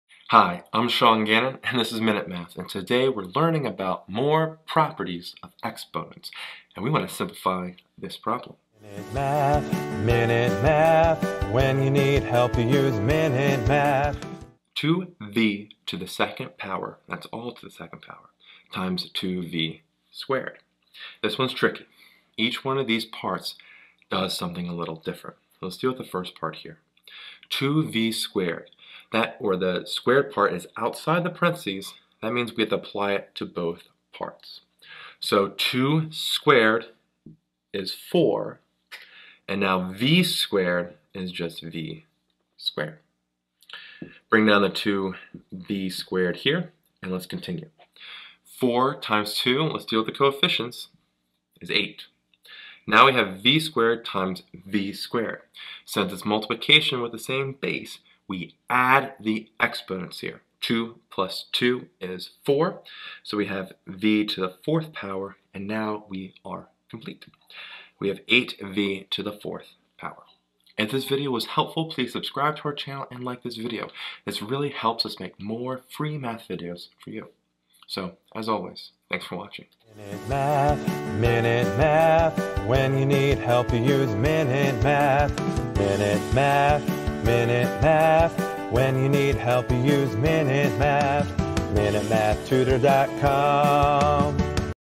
Simplify (2v^2)*2v^2
In this math video lesson we Simplify (2v^2)*2v^2.